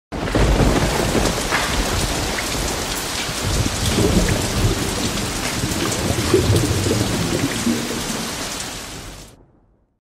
BEI Rain w White Logo
This Rain Logo is a mesmerizing video that focuses all attention on the rain and a blurred image and then your picture or logo materializes right in front of your customer's eyes! All the while you can hear the rain falling and thunder rumbling - THIS IS A GREAT VIDEO TEMPLATE! Everyone needs their picture on one of these!!!